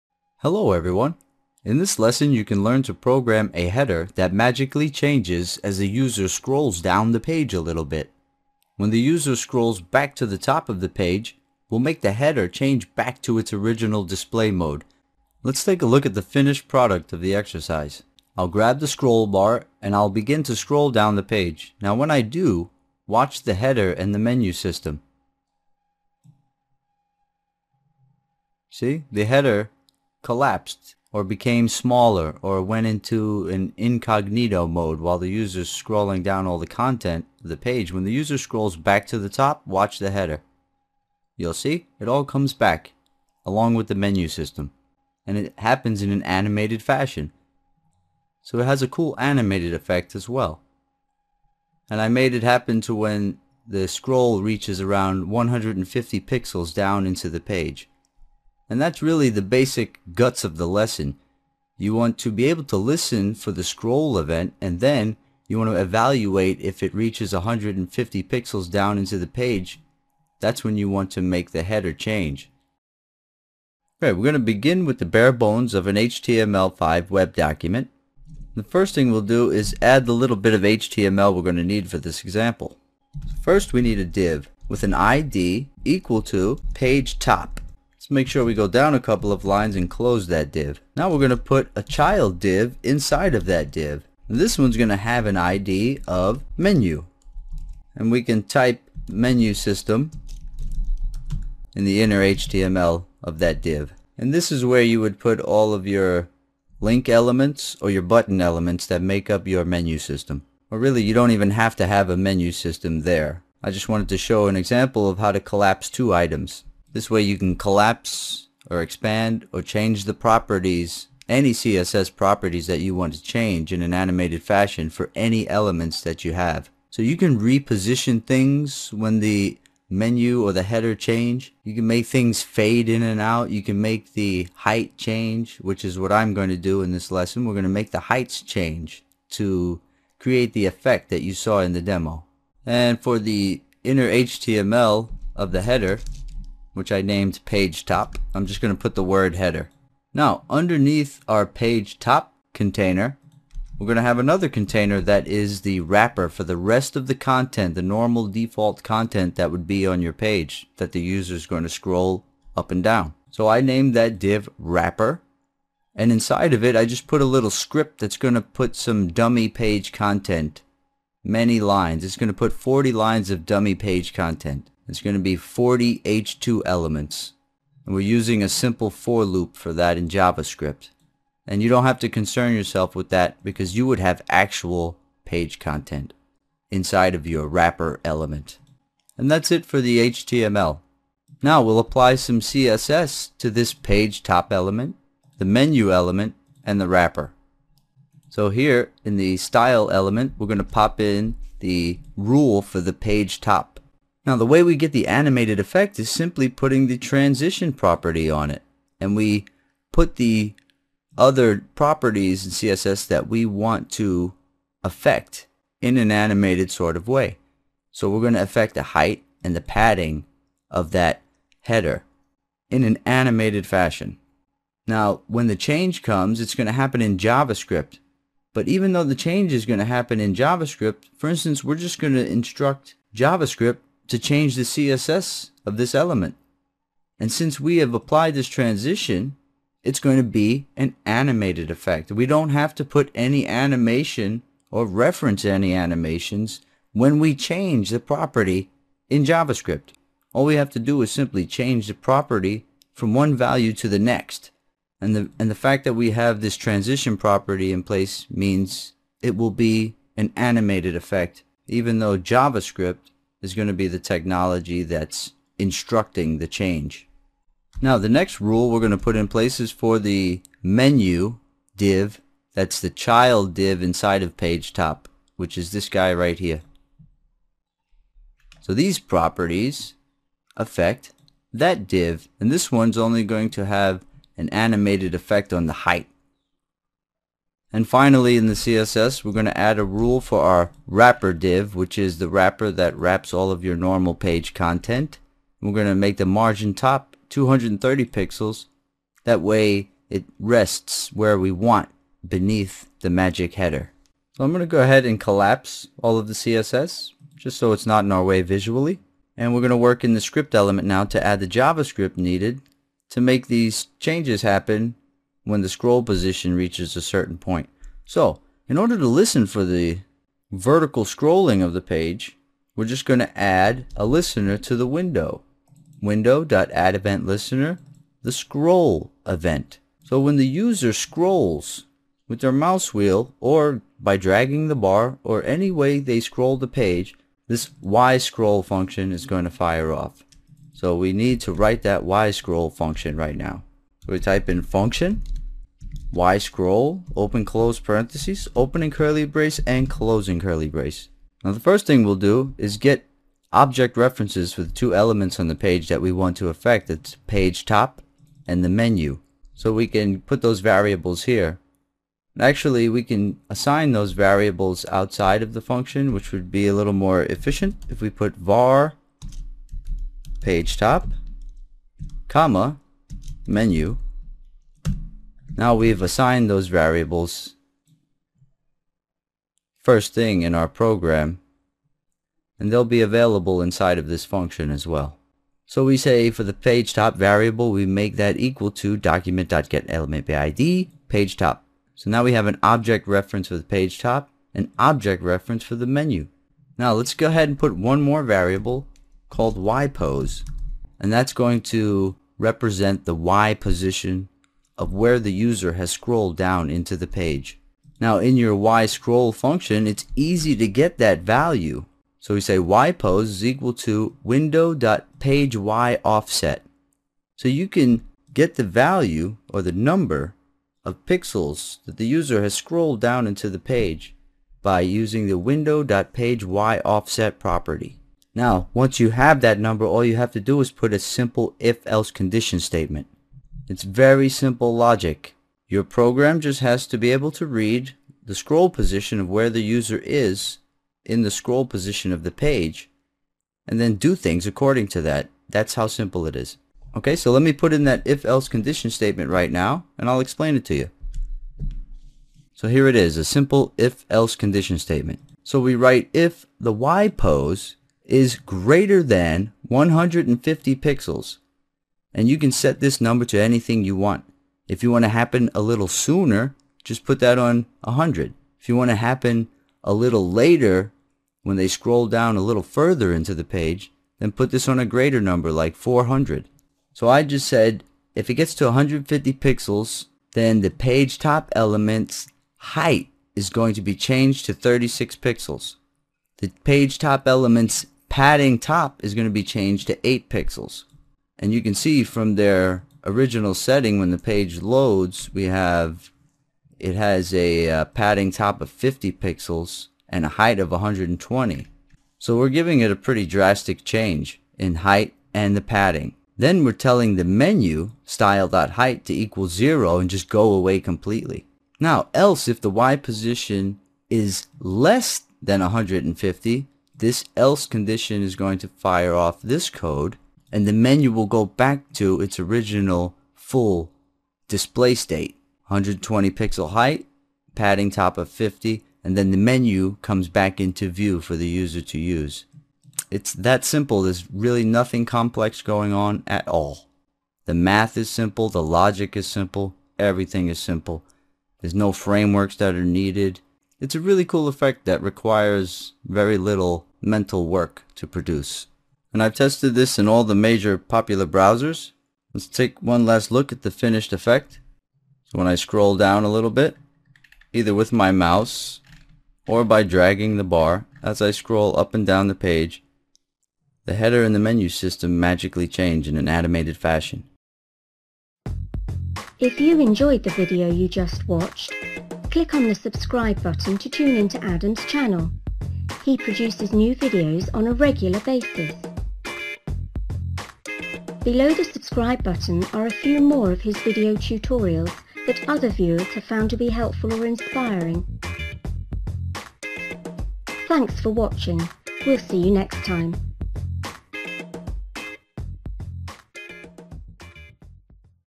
Magic Header Scroll Page Tutorial JavaScript CSS HTML
Learn to program a header that magically changes as the user scrolls down the page. When the user scrolls back to the top of the page we will make the header change back to its original display settings.